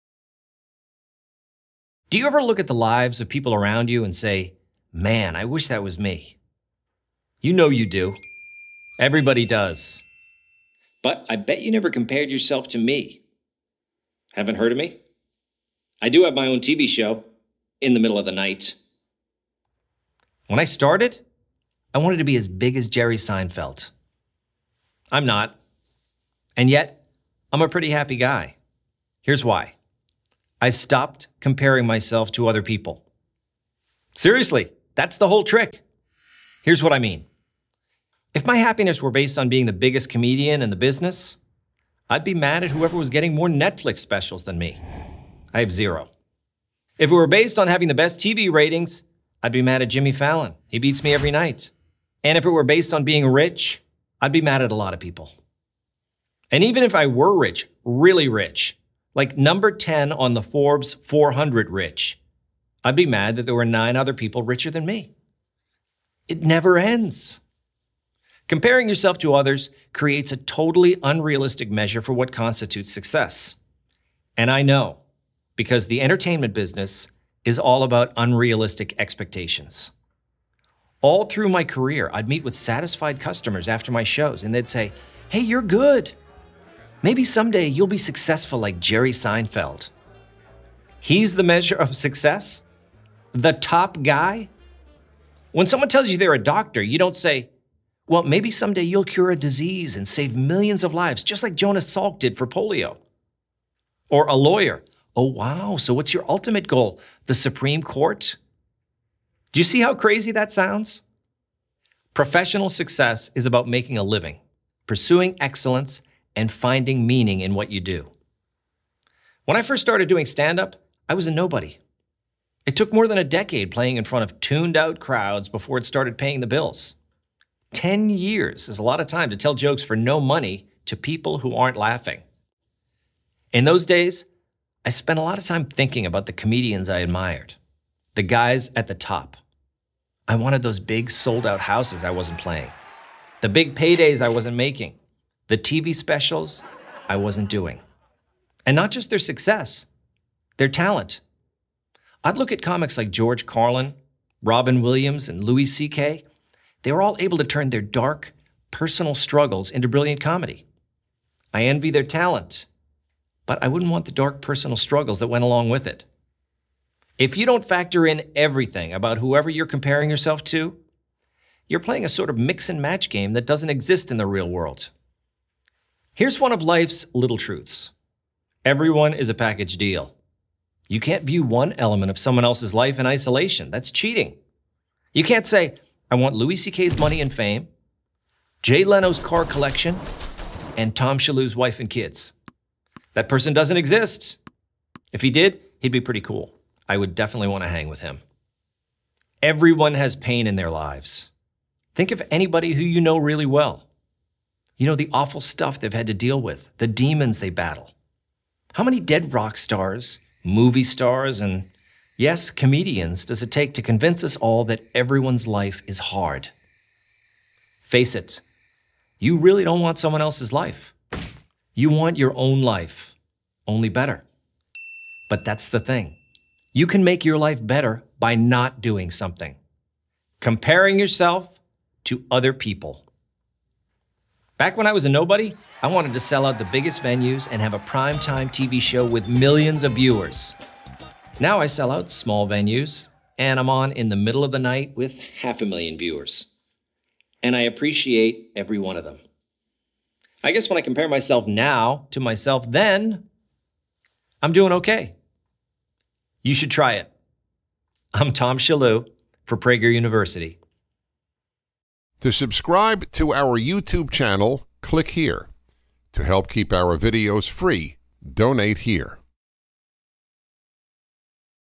Don't Compare Yourself to Others | 5 Minute Video
Have you ever envied someone else's success? Do you sometimes wish you had another person's life? Well, comedian Tom Shillue explains why comparing yourself to other people will put you on the fast track to an unhappy life.

Have you taken the pledge for school choice?

JOIN PragerFORCE!

Script:

Do you ever look at the lives of people around you and say, “Man, I wish that was me!”?

You know you do. Everybody does. But I bet you never compared yourself to me. Haven't heard of me? I do have my own TV show…in the middle of the night.

When I started I wanted to be as big as Jerry Seinfeld.
I'm not. And yet, I'm a pretty happy guy.

Here's why: I stopped comparing myself to other people. Seriously, that's the whole trick. Here's what I mean.

If my happiness were based on being the biggest comedian in the business, I'd be mad at whoever was getting more Netflix specials than me. (I have zero.)

If it were based on having the best TV ratings, I'd be mad at Jimmy Fallon. He beats me every night.

And if it were based on being rich, I'd be mad at a lot of people.
And even if I were rich – really rich, like #10 on the Forbes 400 rich –  I'd be mad that there were nine other people richer than me. It never ends.

Comparing yourself to others creates a totally unrealistic measure for what constitutes success. And I know, because the entertainment business is all about unrealistic expectations.
All through my career I'd meet with satisfied customers after my shows and they'd say,“Hey, you're good! Maybe someday you'll be successful like Jerry Seinfeld.” He's the measure of success? The top guy?

When someone tells you they're a doctor, you don't say, “Well, maybe someday you'll cure a disease and save millions of lives, just like Jonas Salk did for polio.”
Or a lawyer: “Oh, wow, so what's your ultimate goal? The Supreme Court?”

Do you see how crazy that sounds?

Professional success is about making a living, pursuing excellence, and finding meaning in what you do.

When I first started doing standup, I was a nobody. It took more than a decade of playing in front of tuned-out crowds before it started paying the bills. Ten years is a lot of time to tell jokes for no money to people who aren't laughing.

In those days, I spent a lot of time thinking about the comedians I admired. The guys at the top. I wanted those big, sold out houses I wasn't playing. The big paydays I wasn't making. The TV specials I wasn't doing.

And not just their success; their talent. I'd look at comics like George Carlin, Robin Williams and Louis C.K. They were all able to turn their dark, personal struggles into brilliant comedy. I envy their talent, but I wouldn't want the dark personal struggles that went along with it.

If you don't factor in everything about whoever you're comparing yourself to, you're playing a sort of mix-and-match game that doesn't exist in the real world.